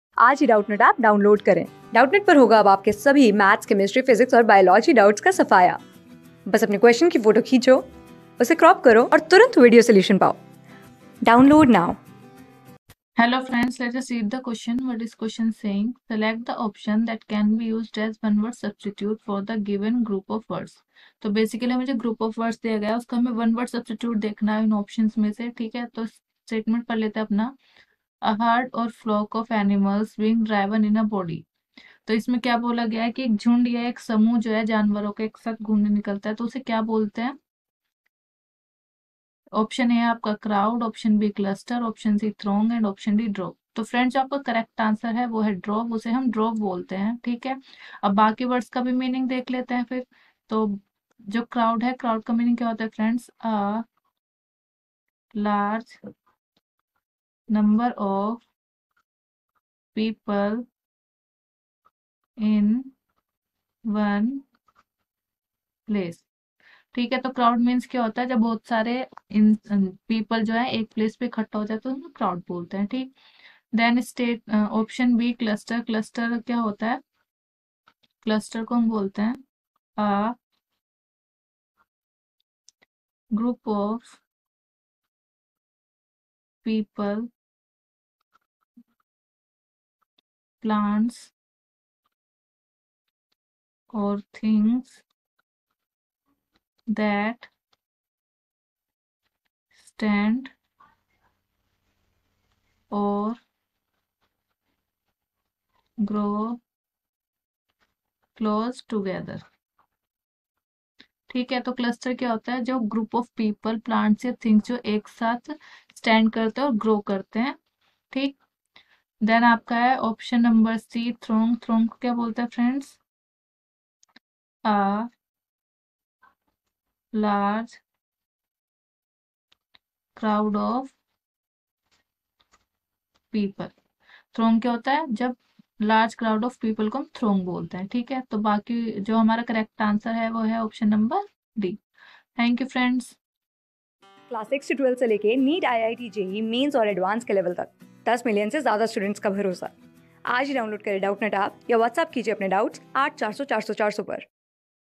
Select the option that can be used as a one-word substitute for the given group of words.A herd ...
Select the option that can be used as a one-word substitute for the given group of words.A herd or flock of animals being driven in a body
Class: 14
Subject: ENGLISH
Chapter: COMBINED GRADUATE LEVEL EXAMINATION 2020 TIER-I
Board:GOVT

👉 Have Any Query? Ask Us.